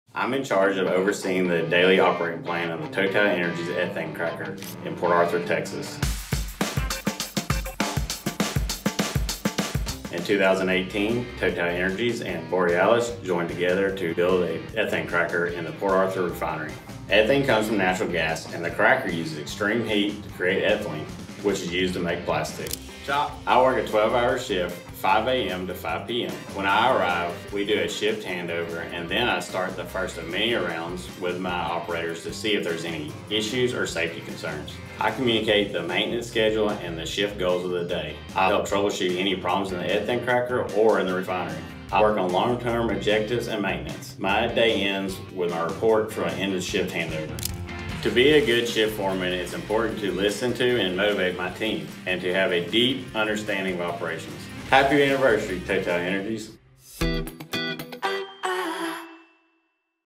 #95 Jacob MARTIN - Petrochemical operations
Meet Jacob Martin, shift foreman on the Ethane Cracker unit at the Port Arthur platform in the United States.
100 years, 100 faces. They continue to write TotalEnergies' history.